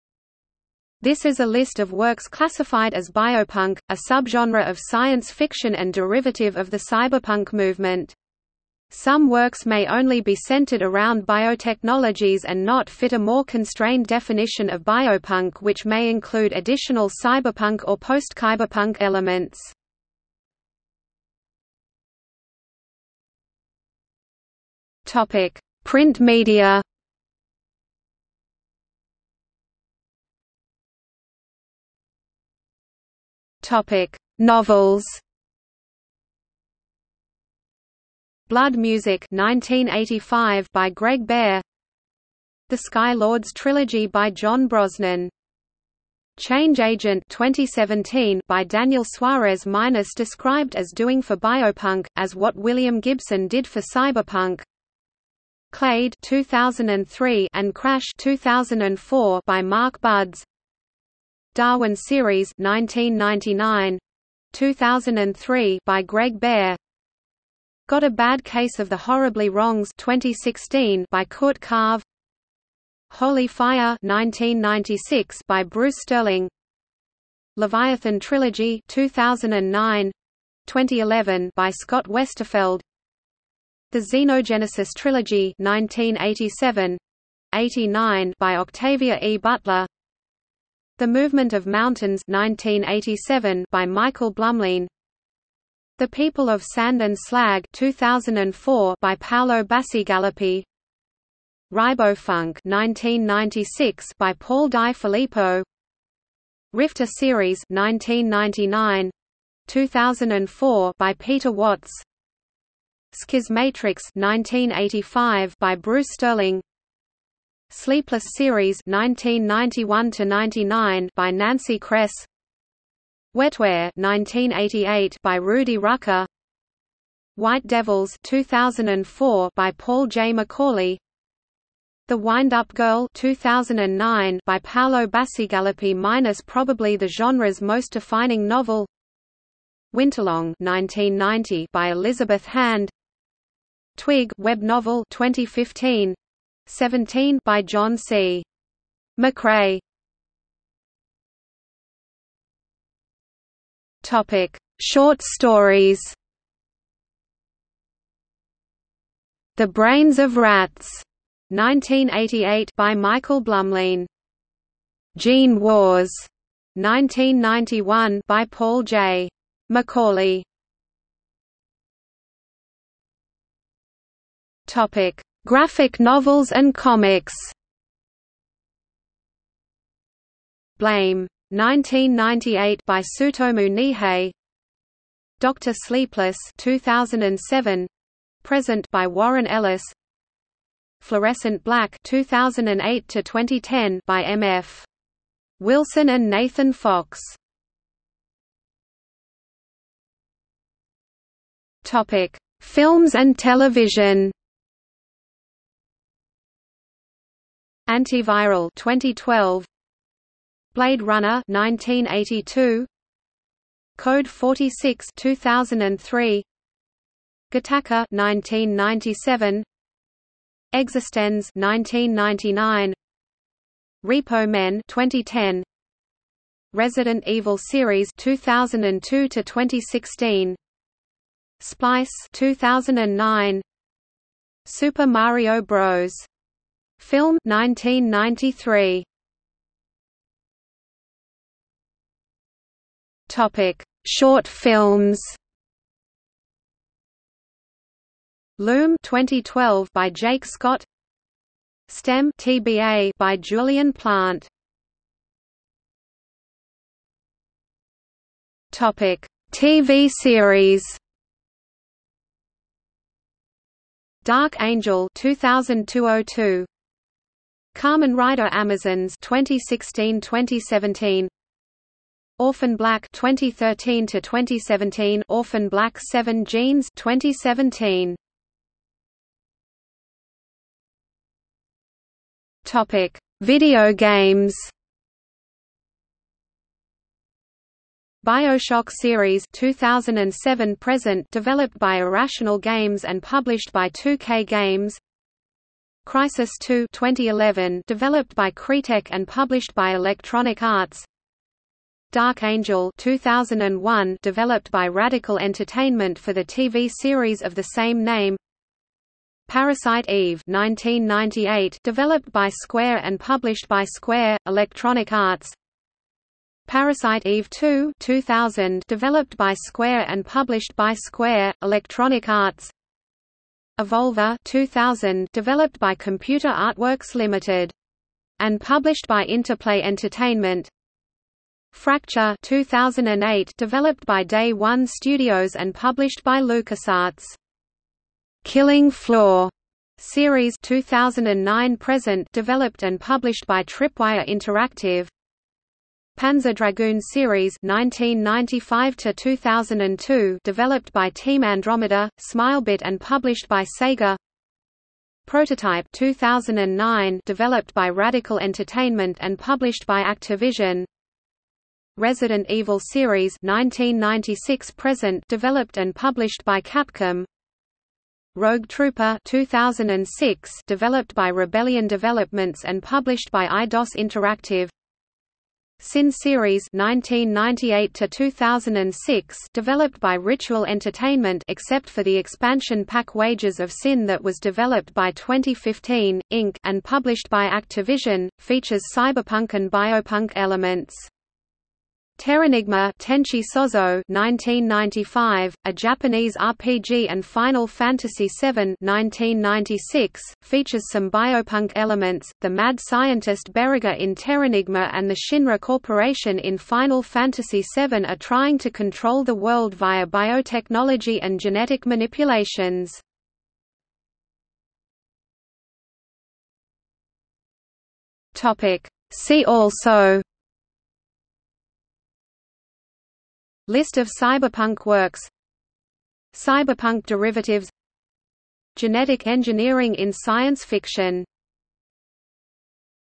List of biopunk works | Wikipedia audio article
00:00:21 1 Print media
00:00:30 1.1 Novels
00:02:30 1.2 Short stories
00:02:51 1.3 Graphic novels and comics
00:03:18 2 Films and television
00:04:00 2.1 Short films
00:04:15 2.2 TV series
00:04:39 3 Video games
00:06:54 4 See also

- Socrates

SUMMARY
This is a list of works classified as biopunk, a subgenre of science fiction and derivative of the cyberpunk movement. Some works may only be centered around biotechnologies and not fit a more constrained definition of biopunk which may include additional cyberpunk or postcyberpunk elements.